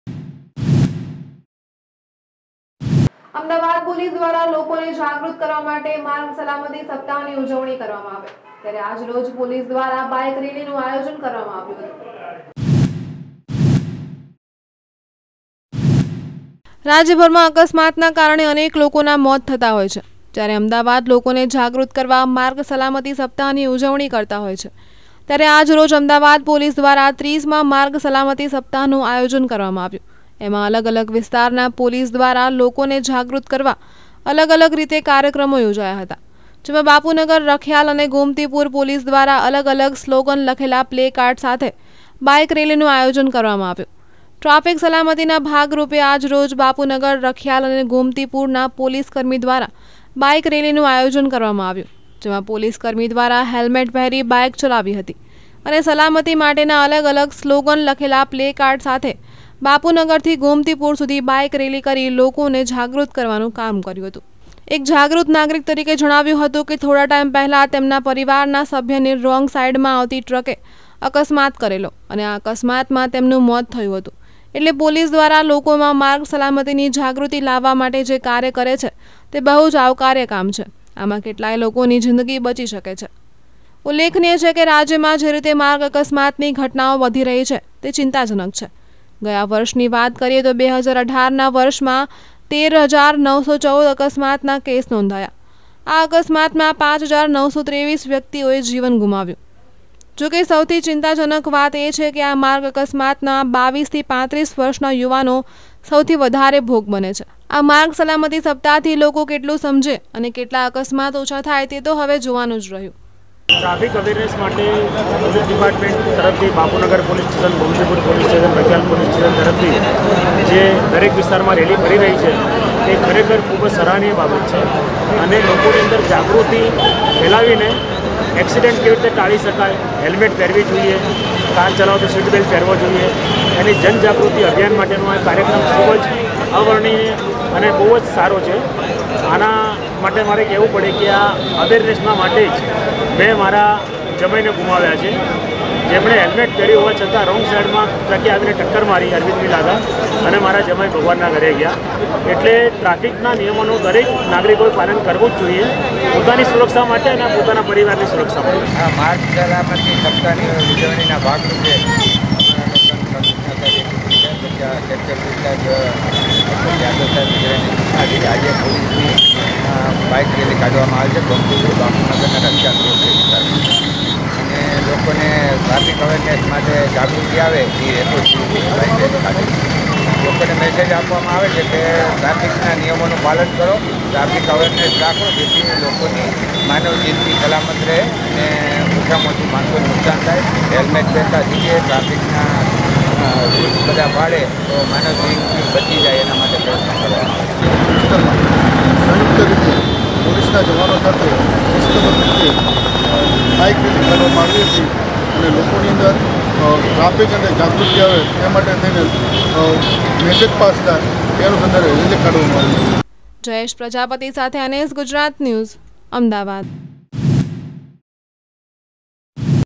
અમદાવાદ માર્ગ સલામતી સપ્તાહ ની ઉજવણી || NS GUJARAT NEWS
અમદાવાદ પોલિસ દ્વારા લોકો ને જાગૃત કરવા માટે માર્ગ સ્લામતી સપ્તાહ ની ઉજવણી કરવામા આવે છે ત્યારે આજ રોજ પોલીસ દ્વારા બાઈક રેલી નું આયોજન કરવામાં આવ્યું